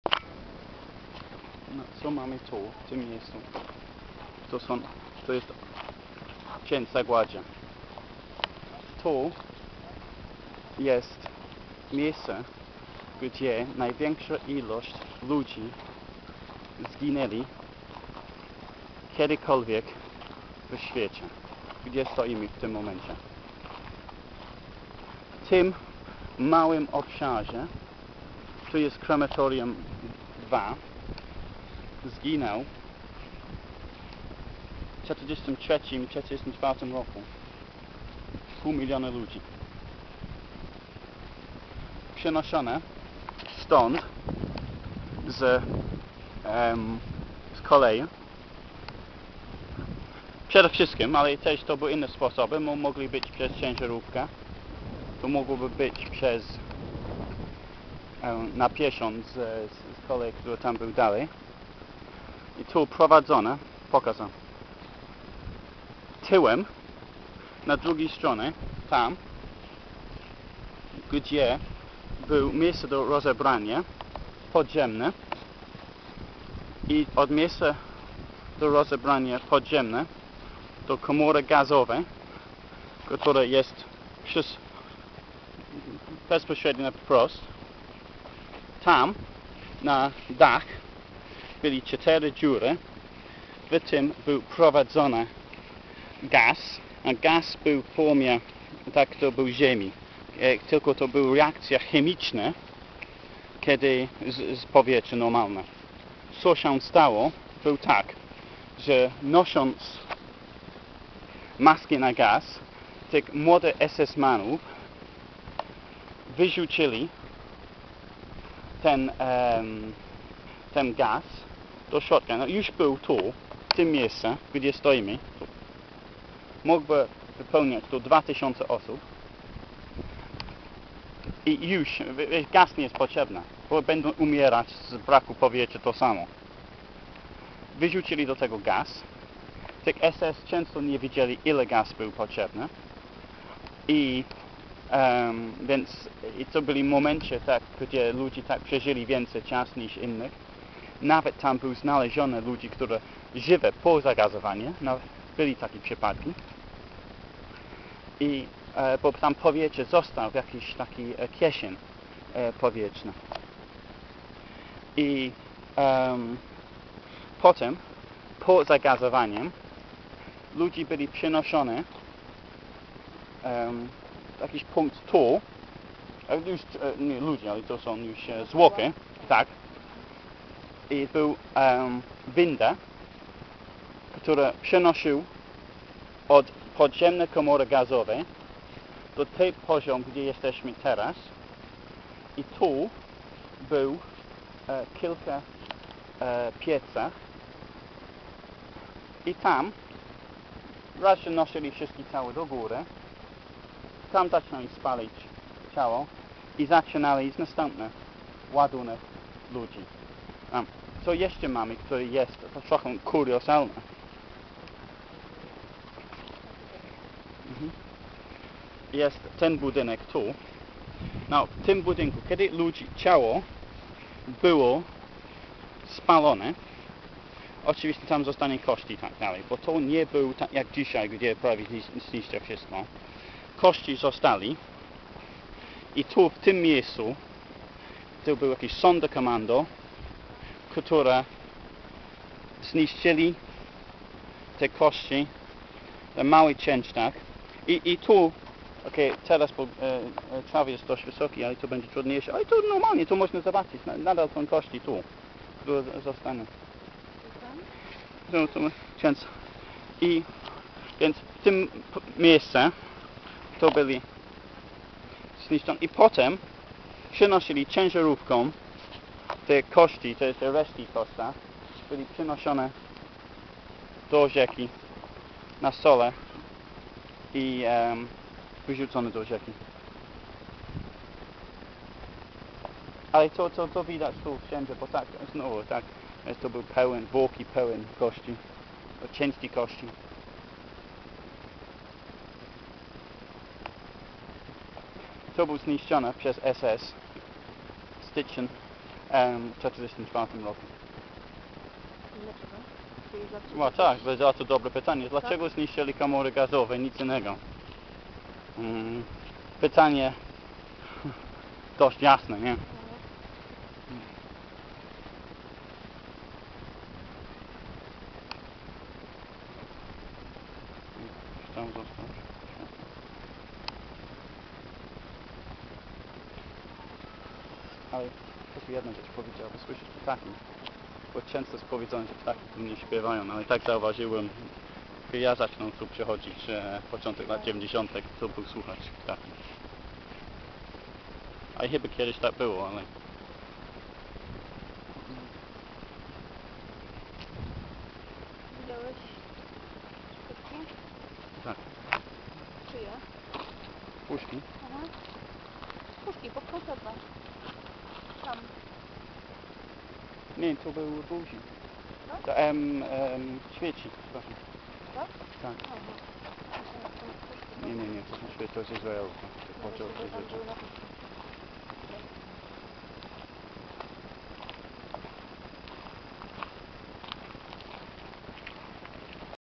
Auschwitz - Birkenau : Krematorium 2
In this small enclosure around 500,000 people were murdered in less than two years in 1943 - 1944.